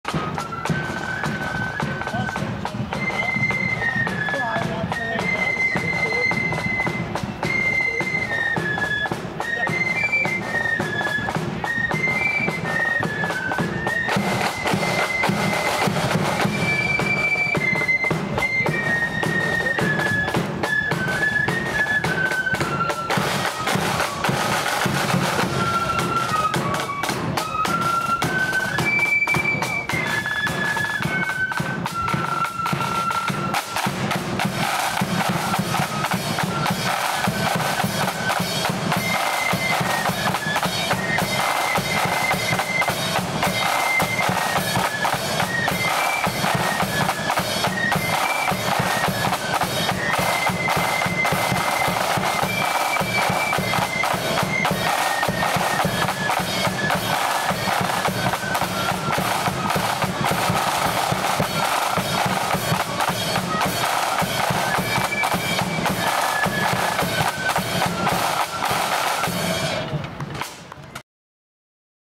Ballycraigy Sons Of Ulster FB @ Upper Falls Protestant Boys FB 40th Anniversary Parade 03/06/23
Ballycraigy Sons Of Ulster Flute Band on parade round the Suffolk Estate in West Belfast for Upper Falls Protestant Boys Flute Band's 40th Anniversary Parade on Saturday 3rd June 2023